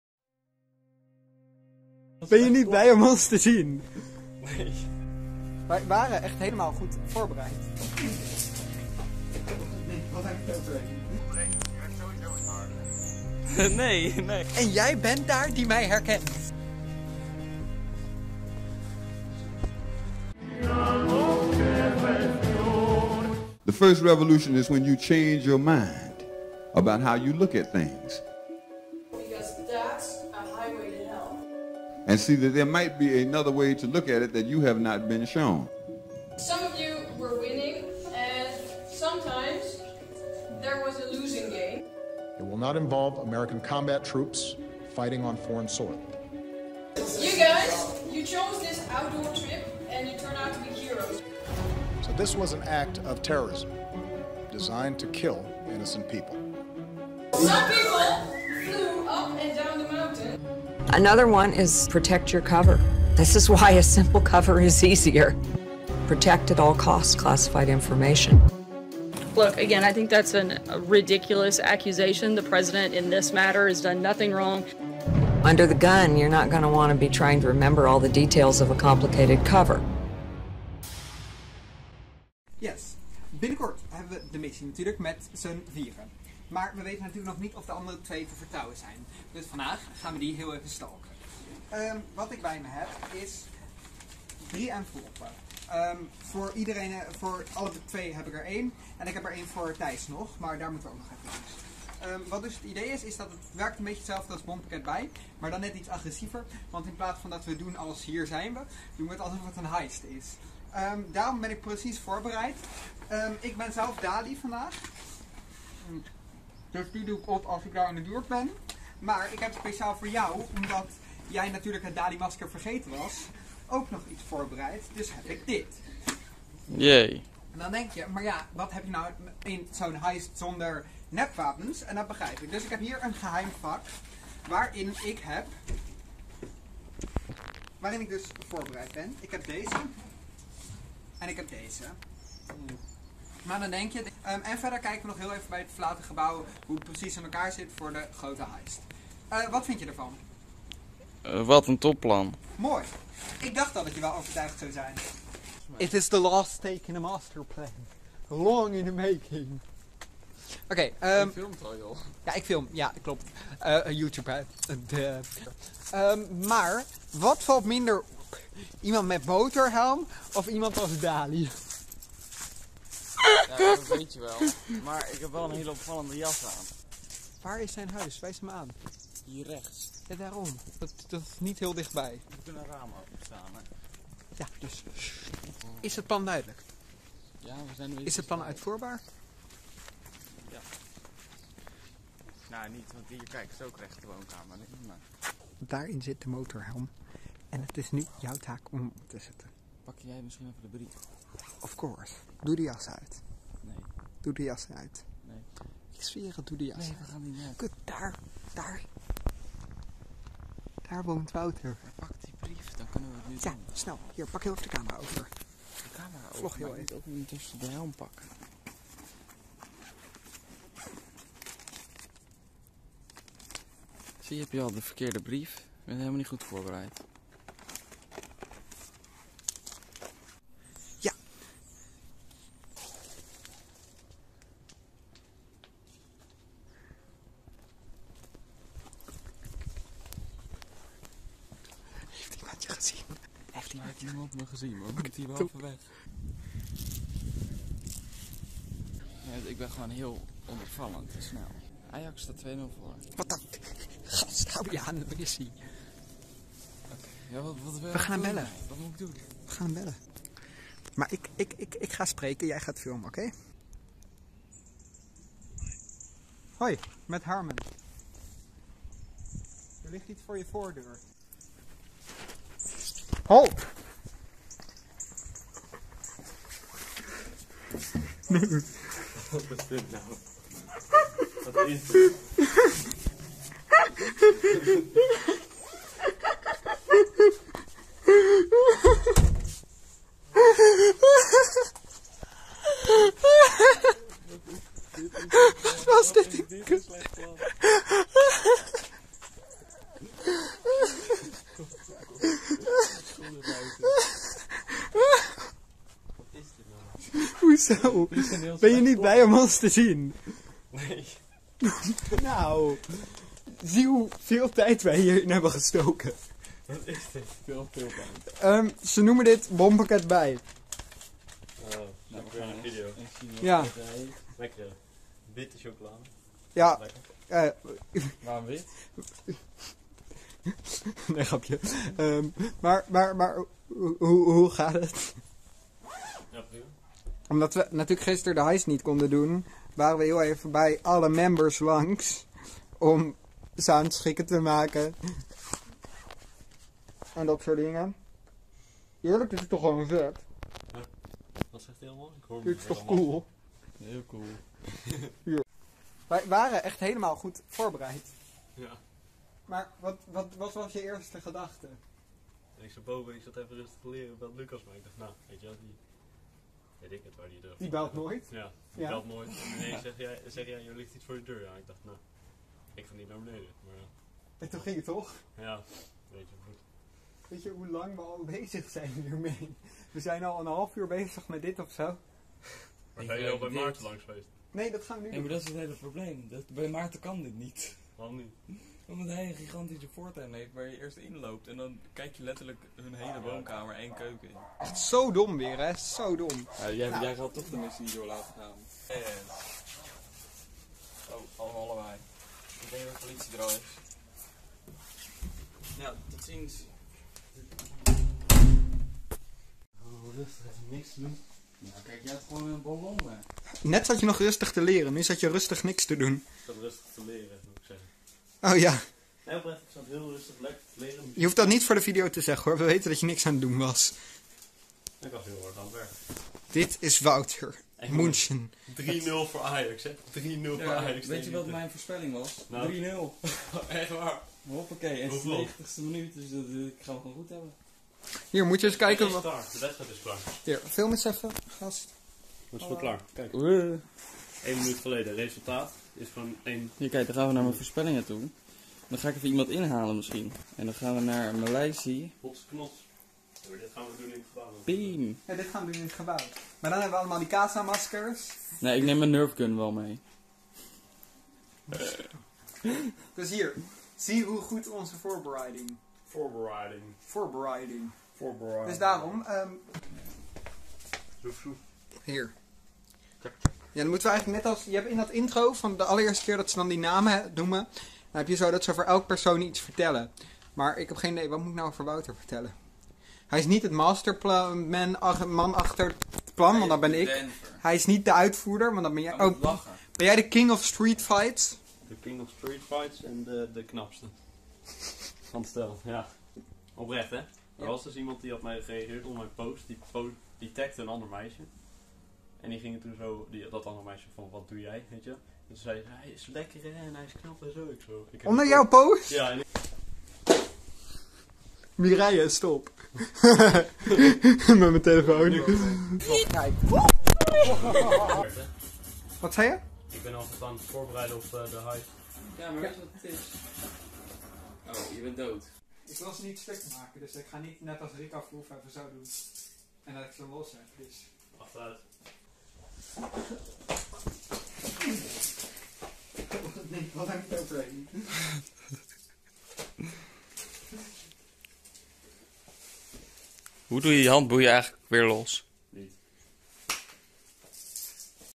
VOORBEREIDING VAN THE HEIST
✔ Hoi, leuk dat je kijkt naar weer een gloednieuwe aflevering van de vlog. In deze miniserie bereiden we voor het allereerst de heist voor. We brengen een bompakket naar Wouter ‘München’ J (@wouter._033) en dat doe ik, Harmen ‘Monaco’ F. samen met Lucas ‘Barcelona’ R. (@lucas_rous_24).

✔ Uploads elke maandag (16:00), woensdag (16:00) en soms vrijdag (16:00) in HD-kwaliteit (720/ 1080 px)